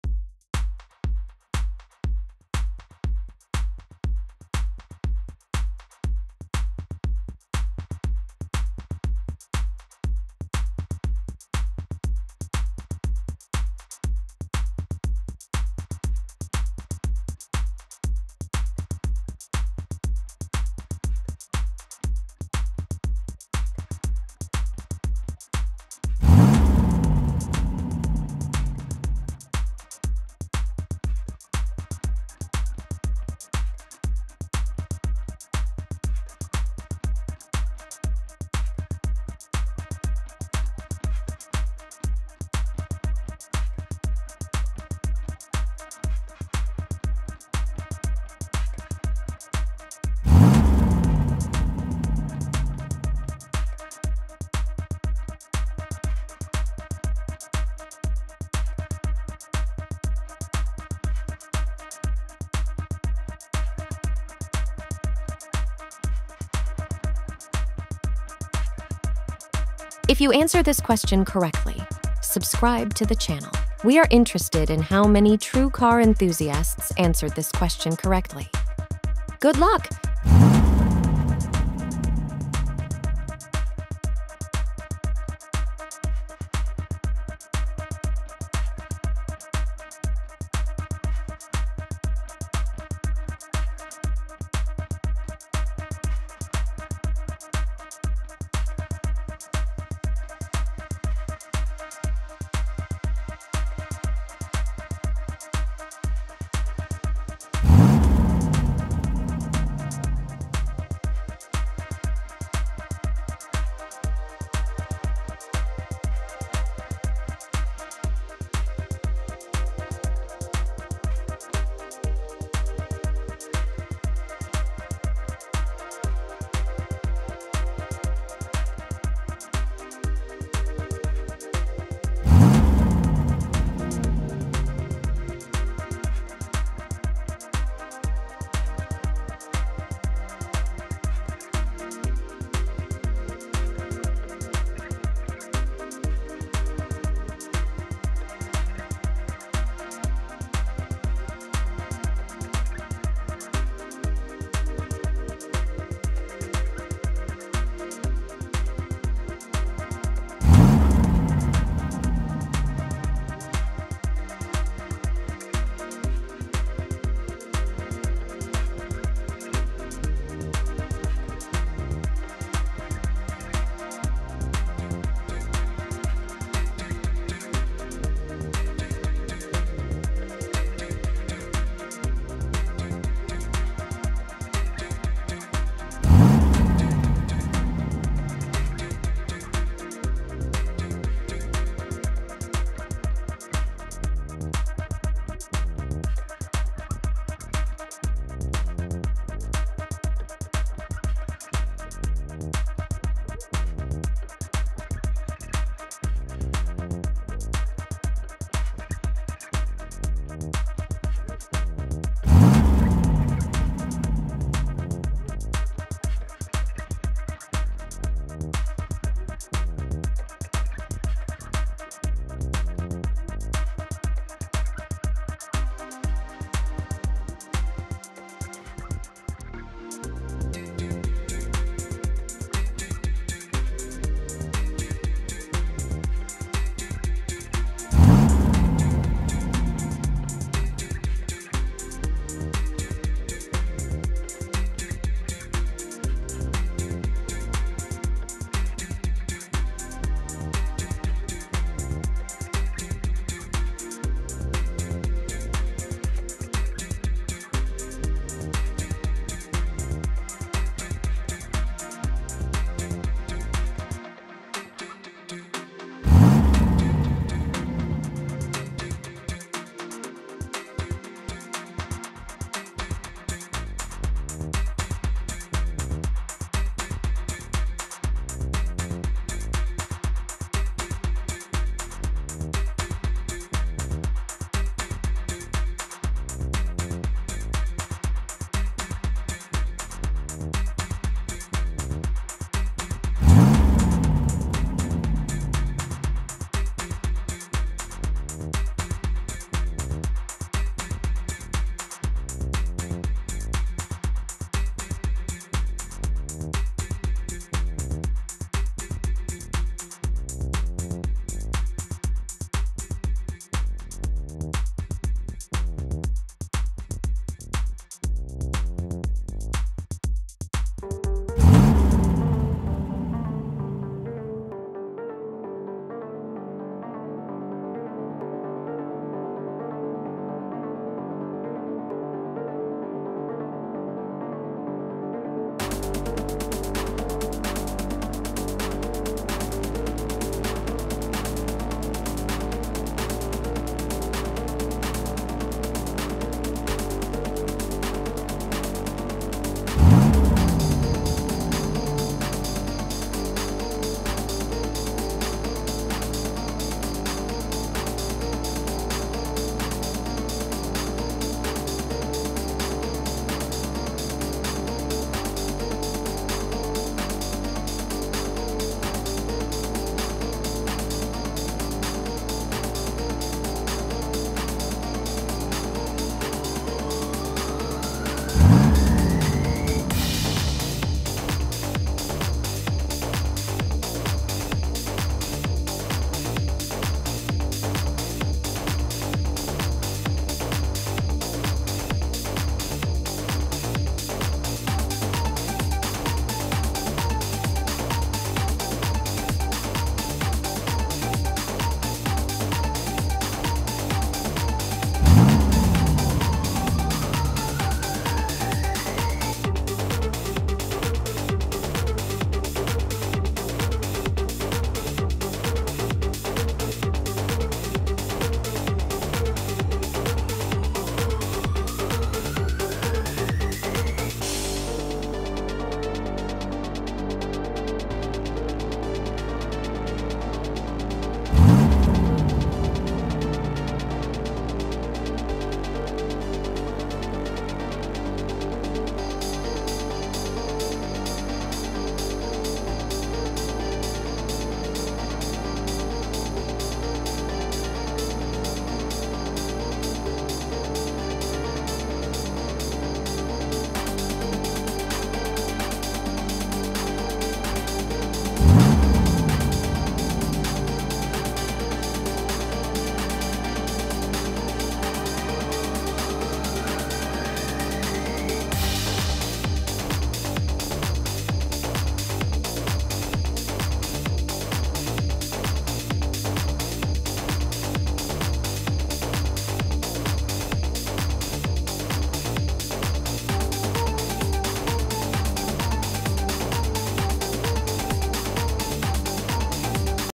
Guess The Car Brand (Famous Cars)
Guess The Car Brand (Famous Cars) 10 seconds to answer comment your results.

Welcome to "Guess The Car Brand (Famous Cars)"! In this exciting quiz video, you'll test your knowledge of car brands and models by guessing which famous cars belong to which brand.

guess the car brand, guess the car quiz, guess the car logo, guess the car name, guess the car by car,